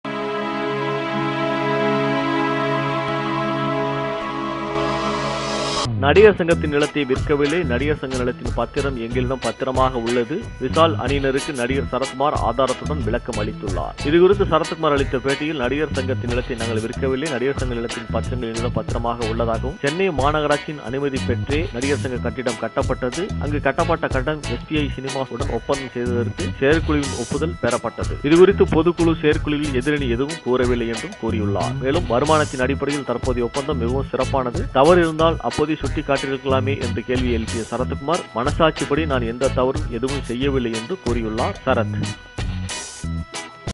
Sarath Kumar With Evidence | Nadigar Sangam Election | Sarathkumar | Vishal - entertamil.com
The South Indian Film Artistes' Association (Nadigar Sangam) elections are coming up on Sunday and the debates are heating up. Actor Sarath Kumar, President, Nadigar Sangam, called for a press meet on Wednesday to issue a clarification on some allegations made by the Pandavar Ani party, led by actor Vishal.

One of the major allegations against Sarath Kumar as President of Nadigar Sangam is that the land which belonged to the union has been sold off to SPI Cinemas for a theatre to be built. The building, where the Nadigar Sangam was housed, was also demolished.

At the press meet, Sarath Kumar stated that the land has not been sold but only been leased to SPI Cinemas. He said that people who are part of the opposing party, the Pandavar Ani, also applauded this move by them. The Nadigar Sangam building was demolished only after they got the relevant demolition order, he stated.

The actor also said that no one raised objections at the time when the Nadigar Sangam entered into the agreement with SPI Cinemas. Sarath Kumar said they would have reconsidered the deal or cancelled the agreement if anyone had approached them, but no one had done so. Things have just turned ugly now, he opined. Sarath Kumar also stated that he was honest, dedicated and sincere.

Earlier, there was talk that the present Nadigar Sangam leaders wanted a compromise with the Pandavar Ani, who were standing against them in the elections. However, as of now, it looks like the elections will take place as scheduled on Sunday.